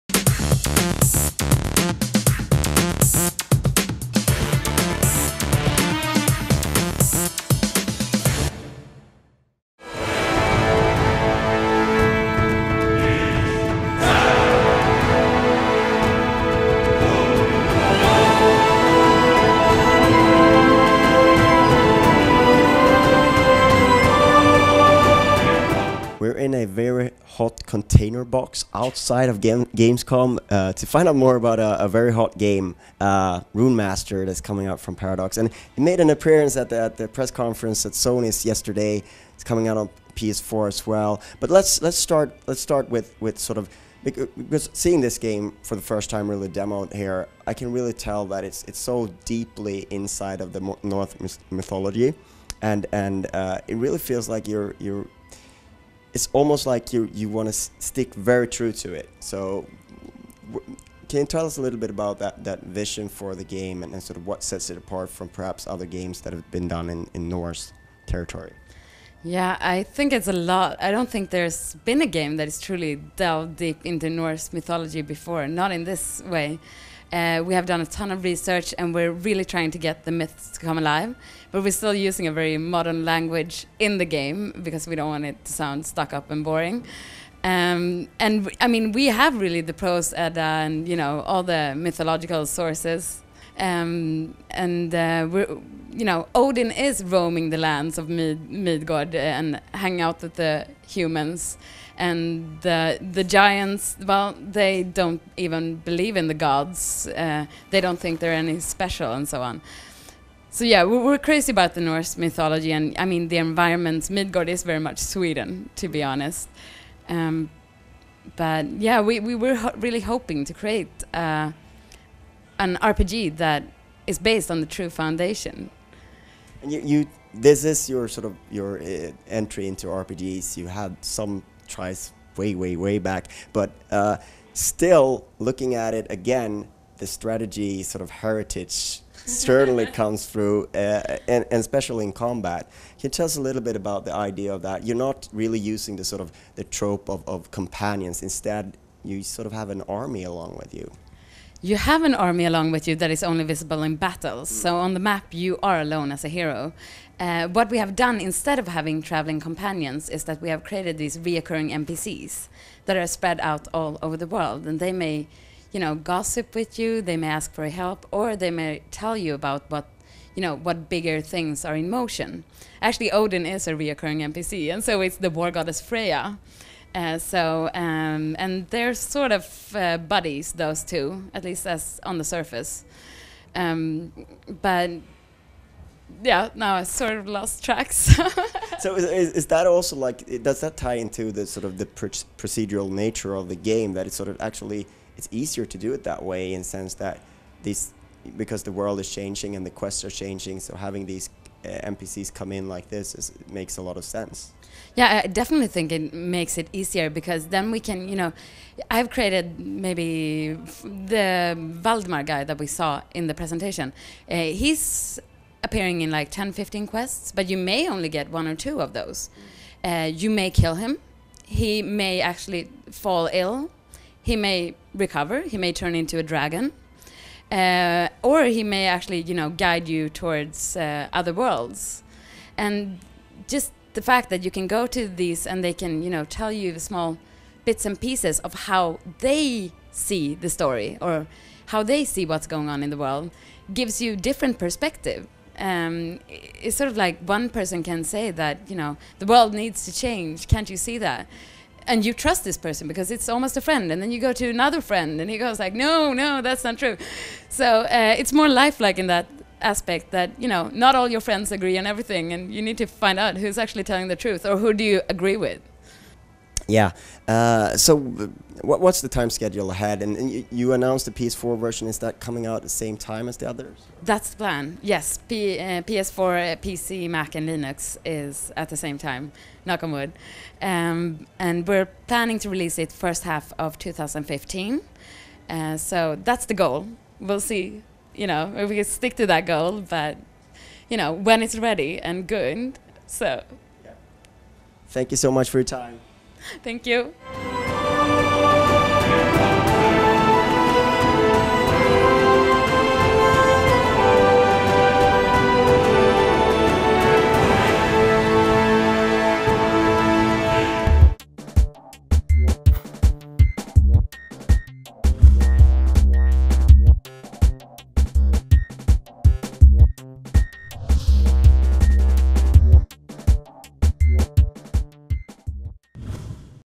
Runemaster - Game Writer Interview
We snagged a couple of minutes with Boel Bermann, Game Writer at Paradox Development Studio and talked about Norse mythology, RPG, characters, and more.